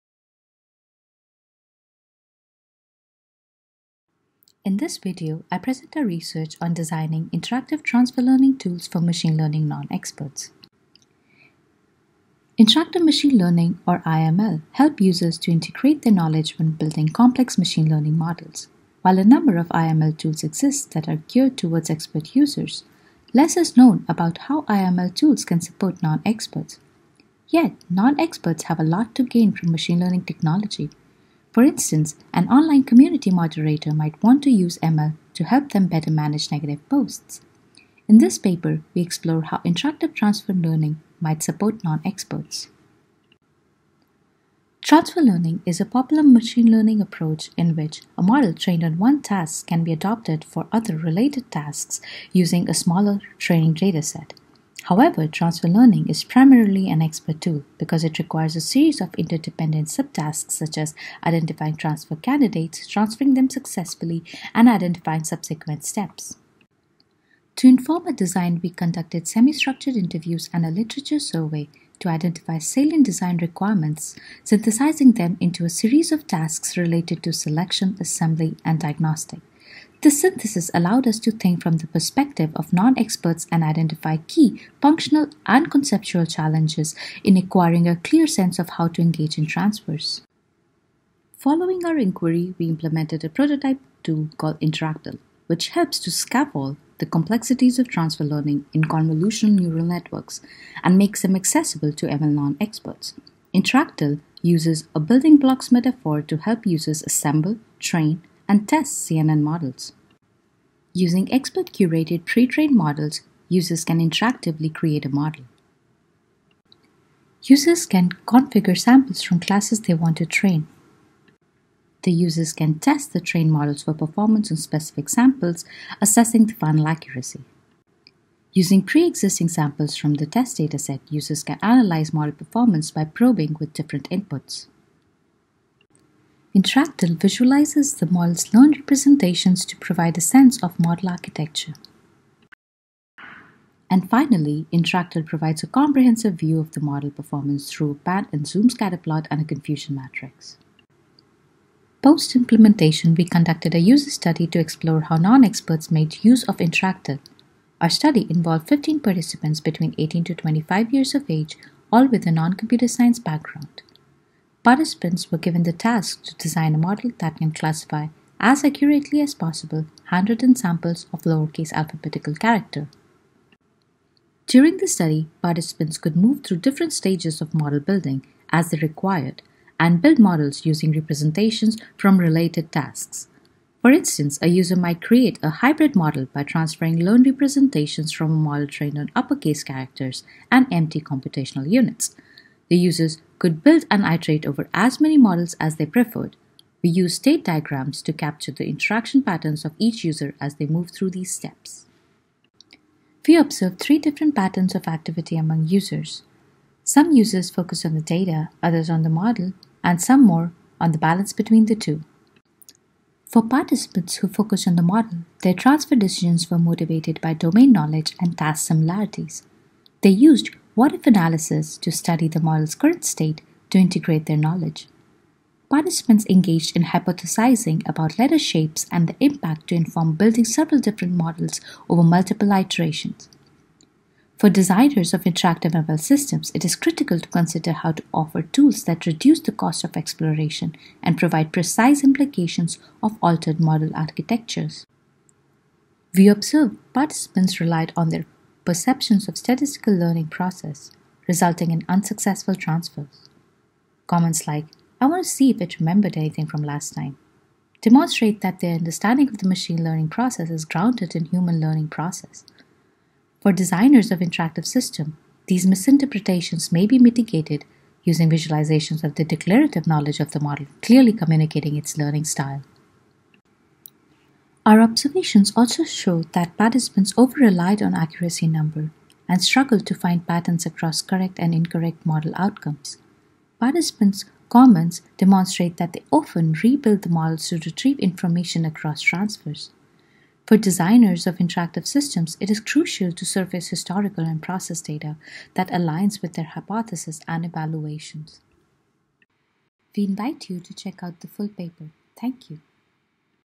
Designing Interactive Transfer Learning Tools for ML Non-Experts
Swati Mishra, Jeffrey M Rzeszotarski

Session: UX and Interaction Design and Research: Techniques, Insights & Prototyping / Reflection, Behavior, Change & Learning

Abstract
Interactive machine learning (iML) tools help to make ML accessible to users with limited ML expertise. However, gathering necessary training data and expertise for model-building remains challenging. Transfer learning, a process where learned representations from a model trained on potentially terabytes of data can be transferred to a new, related task, offers the possibility of providing "building blocks" for non-expert users to quickly and effectively apply ML in their work. However, transfer learning largely remains an expert tool due to its high complexity. In this paper, we design a prototype to understand non-expert user behavior in an interactive environment that supports transfer learning. Our findings reveal a series of data- and perception-driven decision-making strategies non-expert users employ, to (in)effectively transfer elements using their domain expertise. Finally, we synthesize design implications which might inform future interactive transfer learning environments.